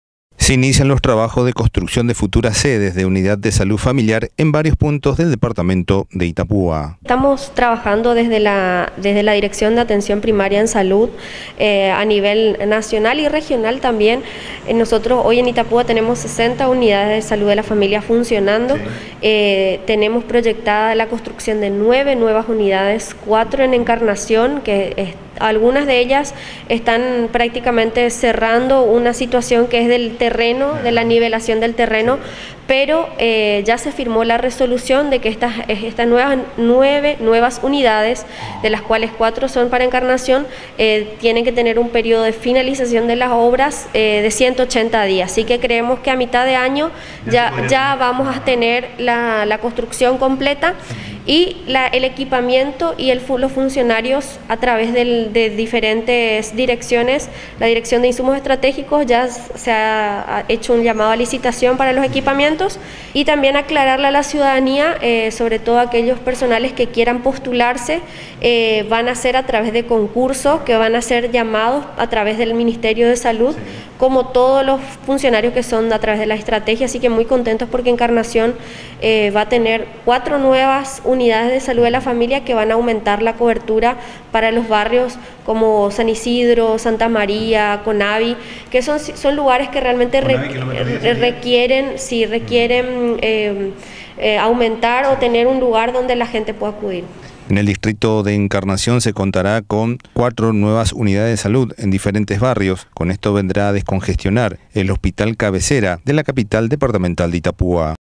INICIA CONSTRUCCIÓN DE 9 UNIDADES DE SALUD FAMILIAR EN ITAPÚA
Así lo confirmó la Dra. Romina Sanabria quien manifestó que el Departamento actualmente cuenta con 60 unidades de salud y ya se encuentran en marcha las nueve nuevas edificaciones.

Cinco serán construidos en la ciudad de Encarnación, la responsable de salud en la región aseguró que desde la cartera sanitaria contratarán funcionario de blanco para las atenciones a toda persona que llega a estos puestos.

Se estima que las obras culminarían pasado el mes de junio.